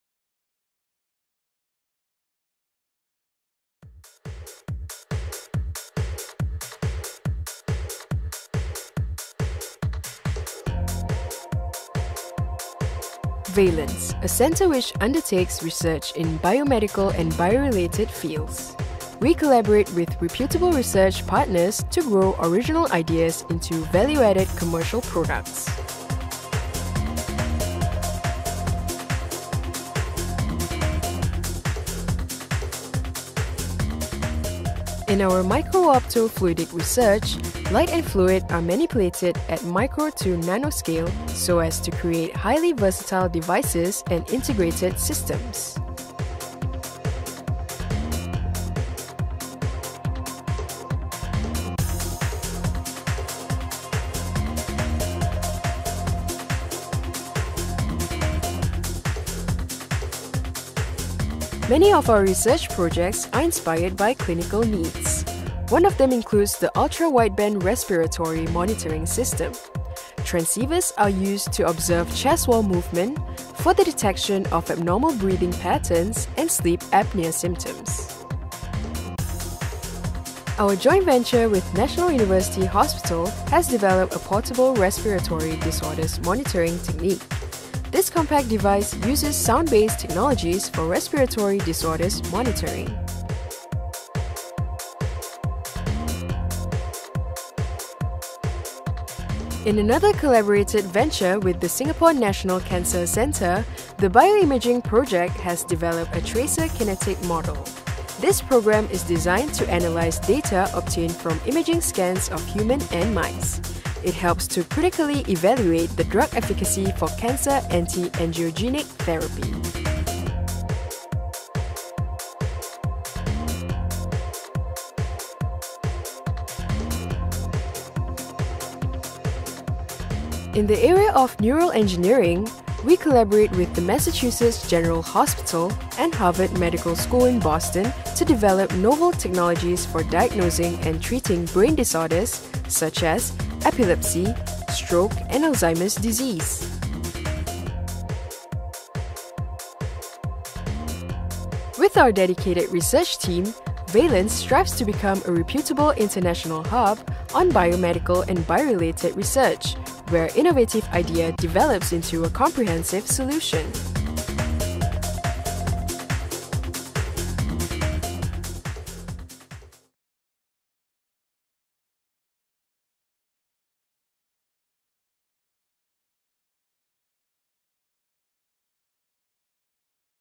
VALENS, Centre of Excellence for Bio-Instrumentation, Devices & Signal Processing
VALENS, a centre which undertakes research in biomedical and bio-related fields

We collaborate with reputable research partners to grow original ideas into value-added commercial products.

In our Micro-opto-fluidic research, light and fluid are manipulated at micro to nano-scale, so as to create highly versatile devices and integrated systems.

Many of our research projects are inspired by clinical needs. One of them includes the Ultra- Wideband (UWB) respiratory monitoring system. Transceivers are used to observe chest wall movement for the detection of abnormal breathing patterns and sleep apnea symptoms.

Our joint venture with National University Hospital (NUH) has developed a portable respiratory disorders monitoring technique. This compact device uses sound-based technologies for respiratory disorders monitoring.

In another collaborated venture with the Singapore National Cancer Centre, the Bio-imaging project has developed a Tracer Kinetic model. This program is designed to analyse data obtained from imaging scans of human and mice. It helps to critically evaluate the drug efficacy for cancer anti-angiogenic therapy.

In the area of neural engineering, we collaborate with the Massachusetts General Hospital and Harvard Medical School in Boston to develop novel technologies for diagnosing and treating brain disorders, such as epilepsy, stroke and Alzheimer's disease.

With our dedicated research team, VALENS strives to become a reputable international hub on biomedical and bio-related research, where innovative idea develops into a comprehensive solution.